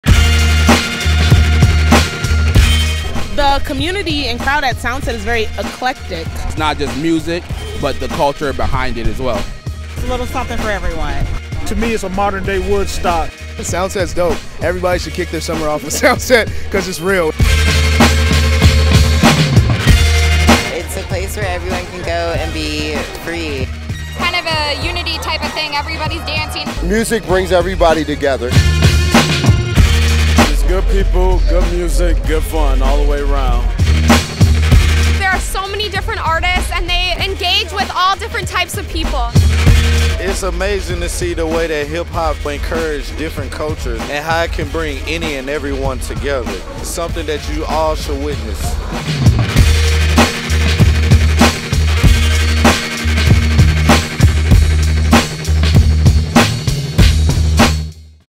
Soundset 2016 - Fan Experience
Fans share some of their favorite parts of Soundset. Join us for 2016's show at the Minnesota State Fair Grounds on May 29th!